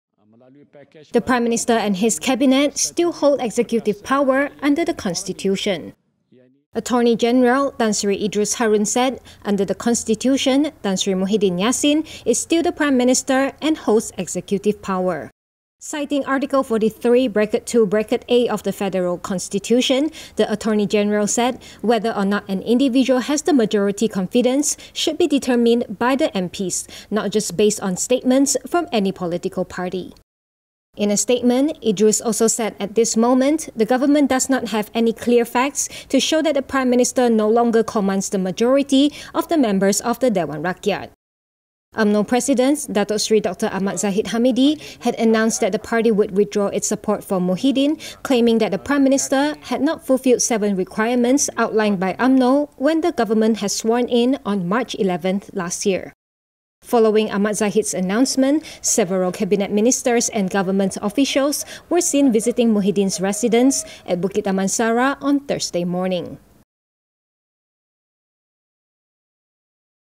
PM and Cabinet still hold executive power, says AG
The Prime Minister and his Cabinet still hold executive power under the Constitution, says Attorney GeneralTan Sri Idrus Harun.